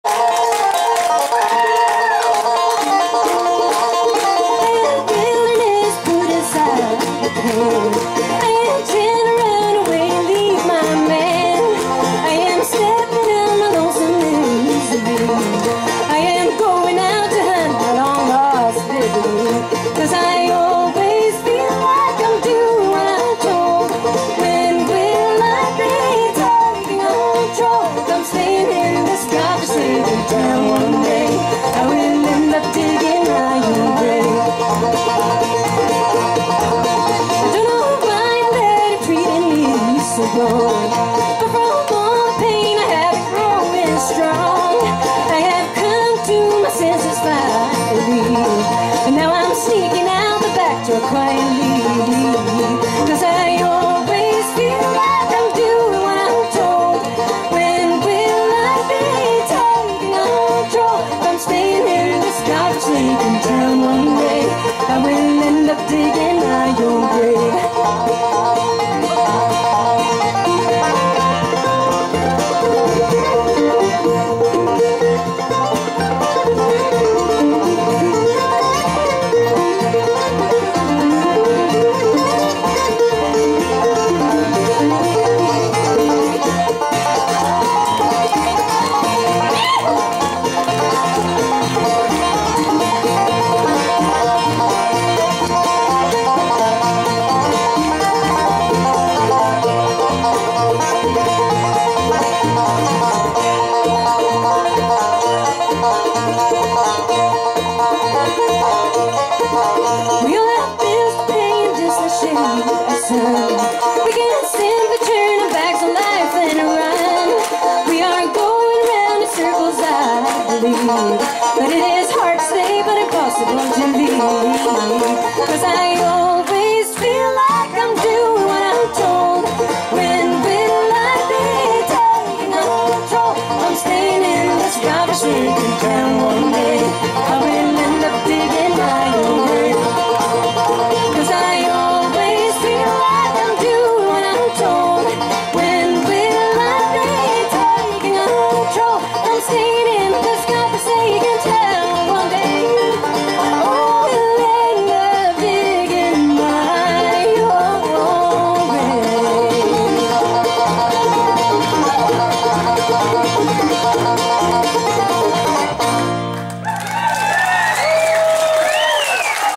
Moonshine Brothers uppträder i Gränna 17 Aug 2013
Med underbara Angelina Darland.
Se mer av Gränna festival 2013 på min kanal. See more videos from Grenna Bluegrass festival 2013 on my channel.